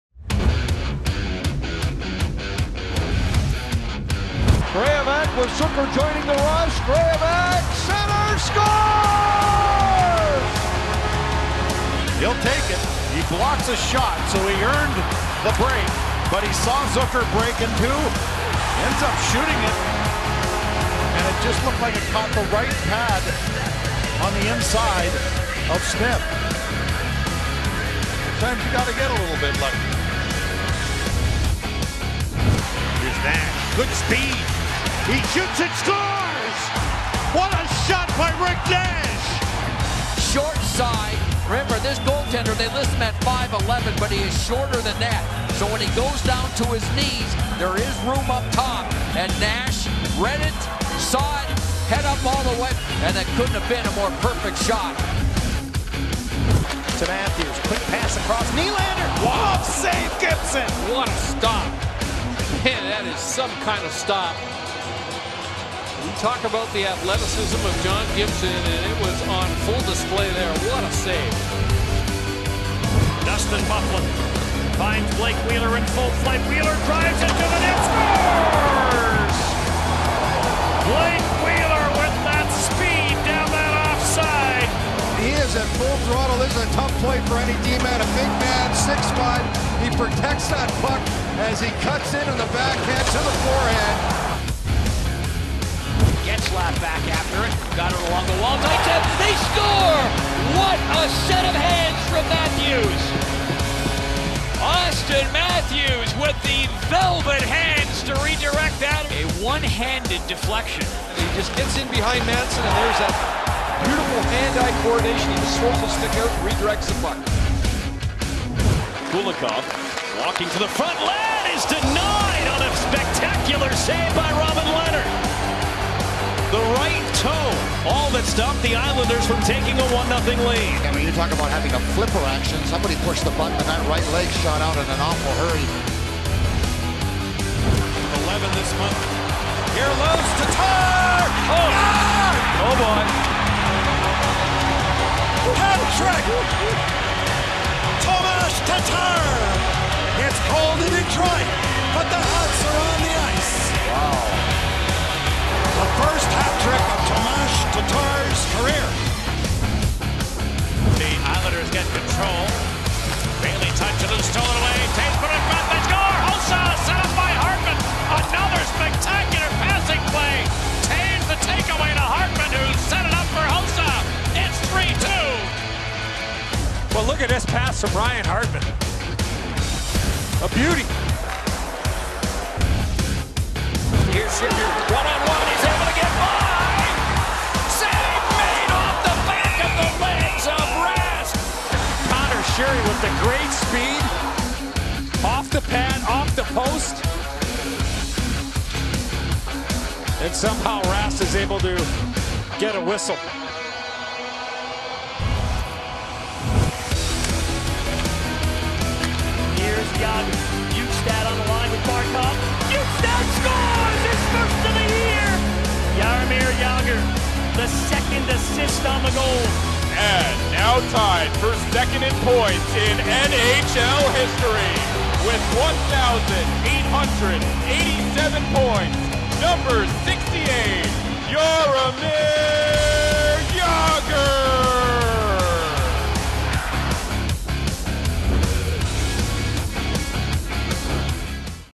Топ-10 моментов недели / Top-10 plays of the week (21 Dec 16)
Top 10 plays from week ten include jaw-dropping saves from Gibson and Rask, one-handed skill from Matthews and a milestone moment from Jagr